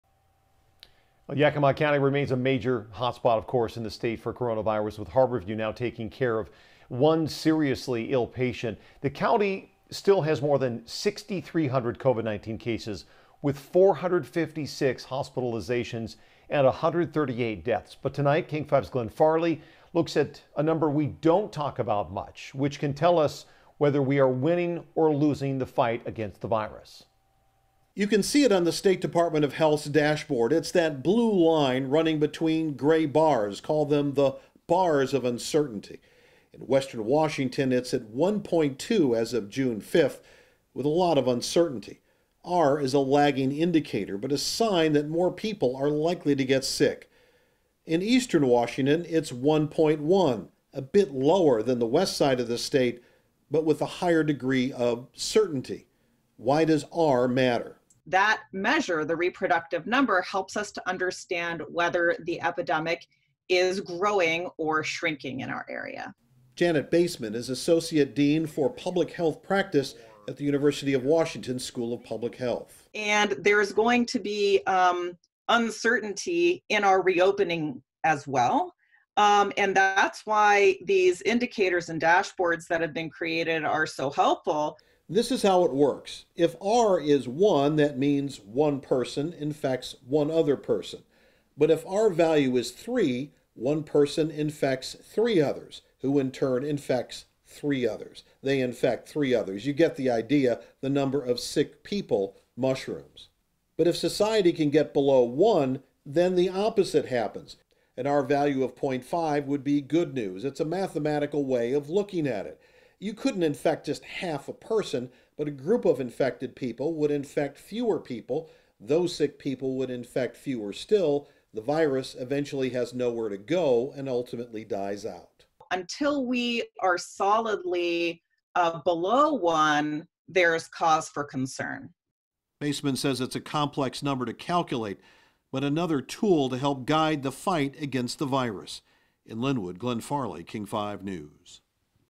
How the 'R' estimate explains the spread of coronavirus in Washington
Aside from cases, hospitalization and tests, the 'R' estimate — or reproductive number — helps show if COVID-19 is growing or shrinking in a community.